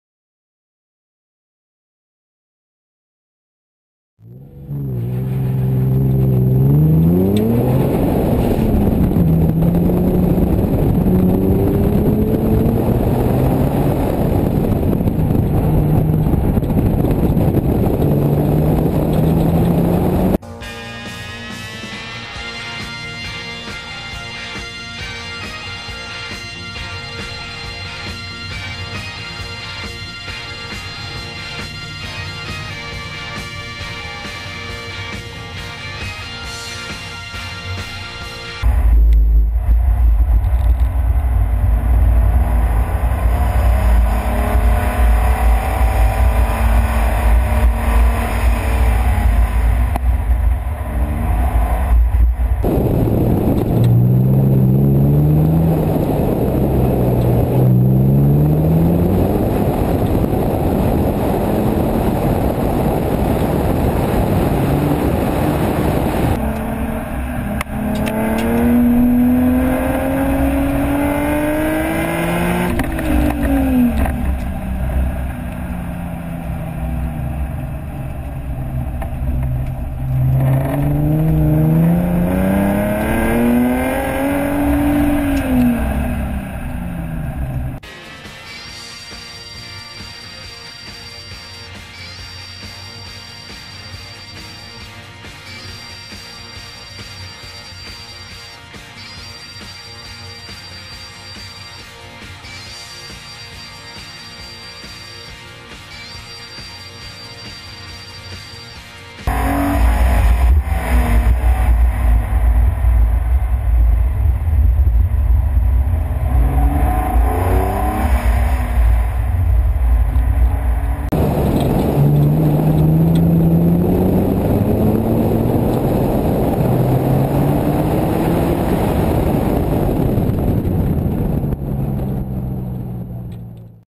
2013 Kitsap Kup 2
Event #2 of the season, my group lucked out and had a dry course.